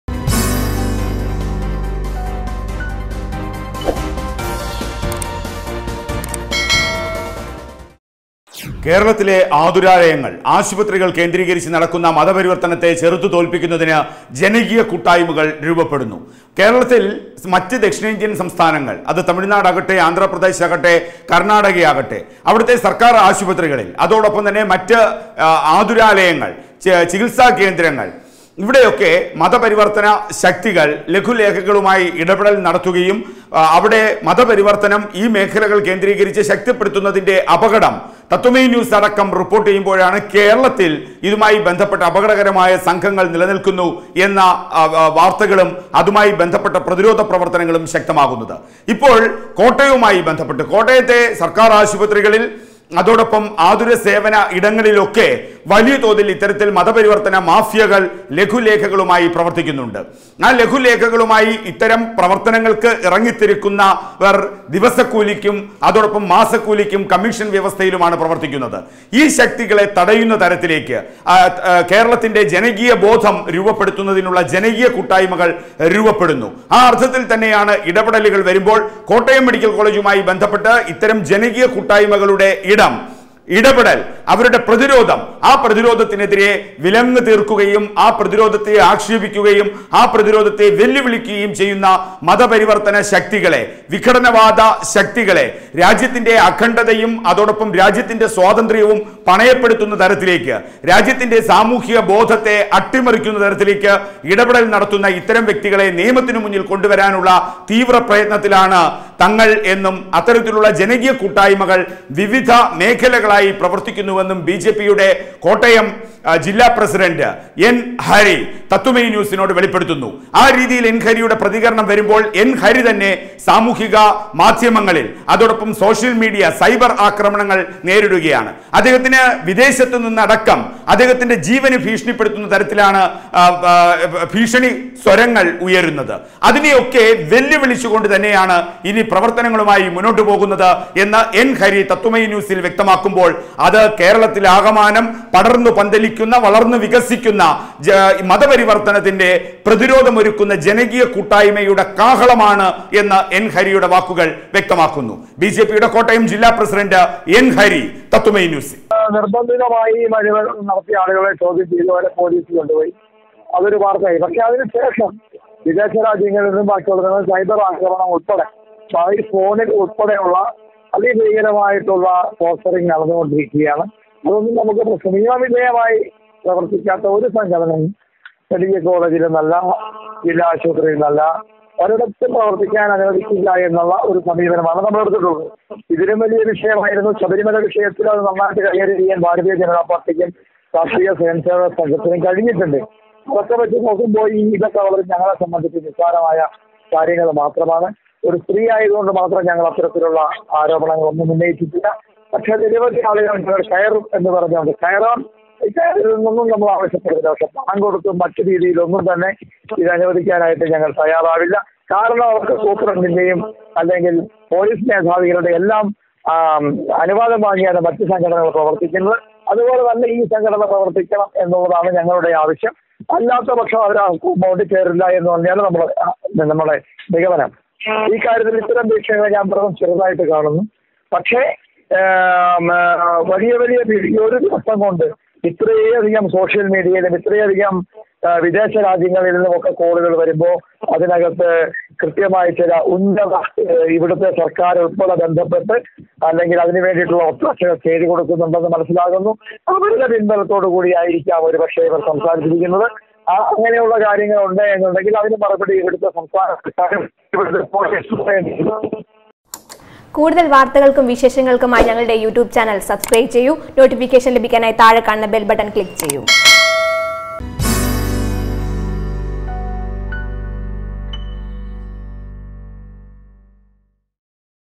ജിഹാദികൾക്കെതിരെ കേരളം ഉണരുന്നു.ജനകീയ കൂട്ടായ്മകൾ ഉദിച്ചുയരും
ജിഹാദികൾക്കെതിരെ കേരളം ഉണരുന്നു. ജനകീയ കൂട്ടായ്മകൾ ഉദിച്ചുയരും. ജീവൻ കൊടുത്തും ജിഹാദി വേരറുക്കാൻ പ്രതിജ്ഞ ചെയ്ത് എൻ ഹരി. കൊല്ലാനെങ്കിൽ കൊന്നോളൂ എന്ന് വെല്ലുവിളിച്ച് ബിജെപി കോട്ടയം ജില്ല പ്രസിഡന്റ് എൻ ഹരി തത്വമയി ന്യൂസിൽ.